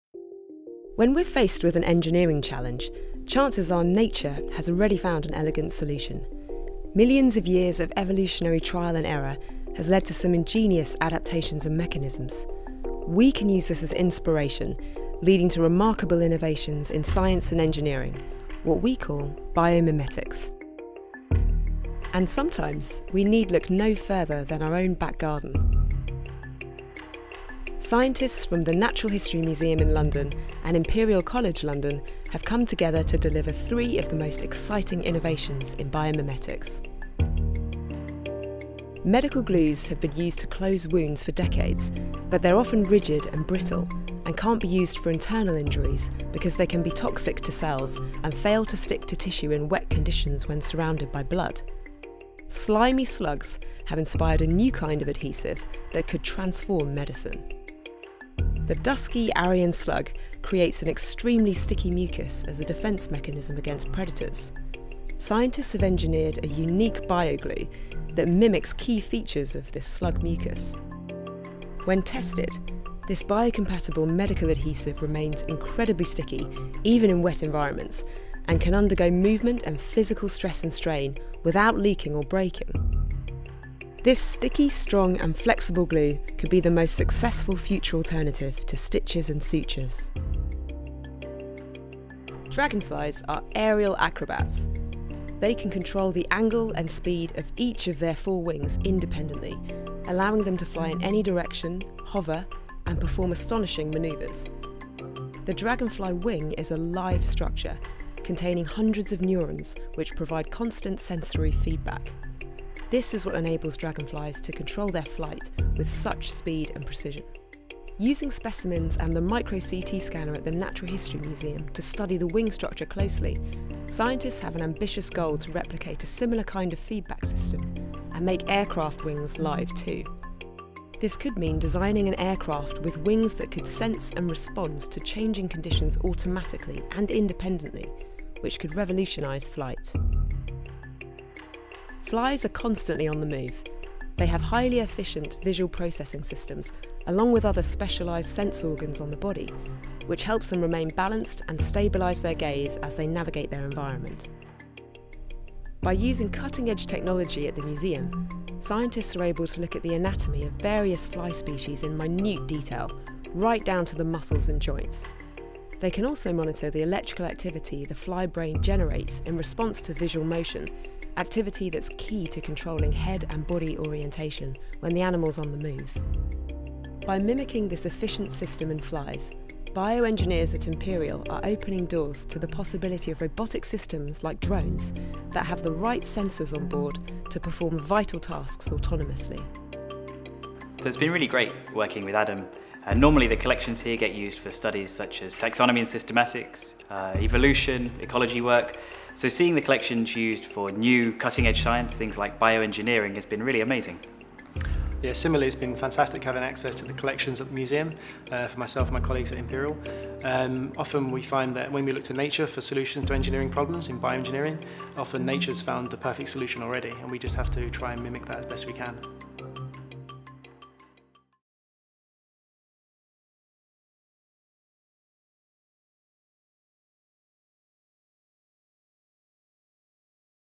How nature is solving our engineering challenges | Natural History Museum
Discover how scientists are taking inspiration from the natural world to solve problems in robotics, medicine and aeronautical engineering.